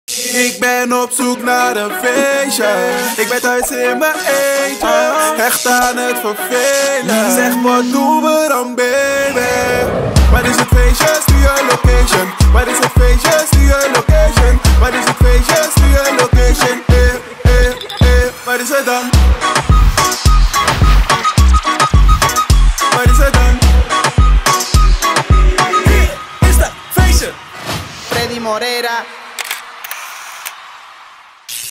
Volop GENIETEN😏 tijdens TILBURGSE KERMIS🍭 - FREDDY MOREIRA
Heerlijk! Volop genoten van de Smèrrig hosting bij de Desperados Roadshow  tijdens de TILBURGSE KERMIS gisteren.. Vergeet niet: BLIJF LOCATION STREAMEN!